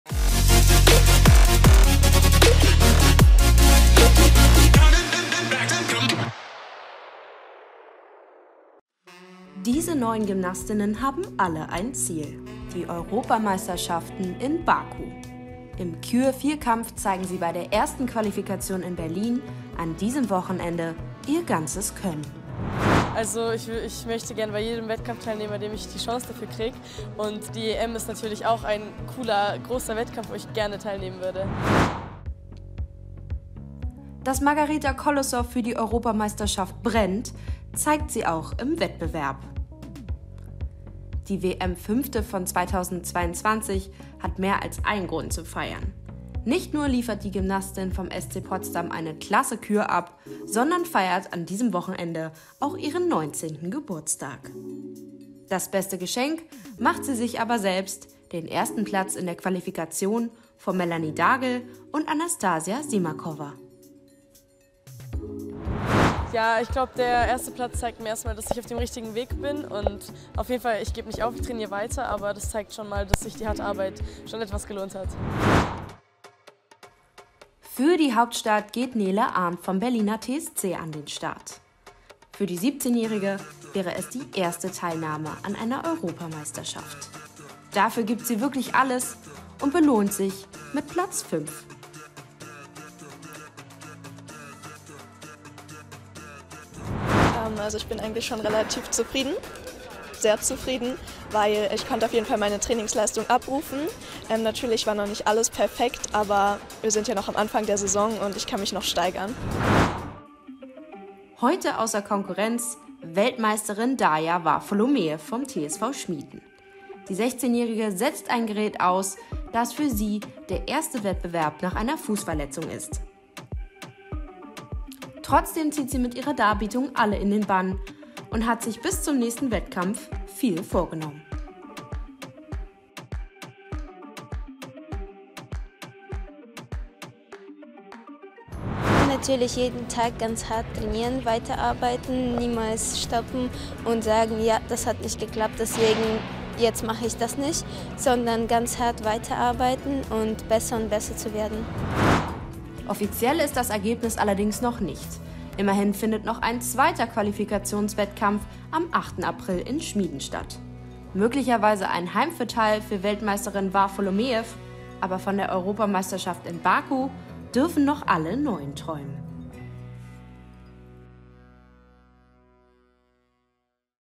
WELTMEISTERIN DARJA VARFOLOMEEV in BERLIN I RSG I EM Qualifikation 2023
Neun Gymnastinnen haben alle ein Ziel: die Europameisterschaften in Baku. Im Kür-Vierkampf zeigen sie bei der 1. Qualifikation in Berlin an diesem Wochenende ihr ganzes Können.
Unter anderem mit dabei: Weltmeisterin Daria Varfolomeev (TSV Schmiden), Margarita Kolosov (SC Potsdam) & Neele Arndt (Berliner TSC).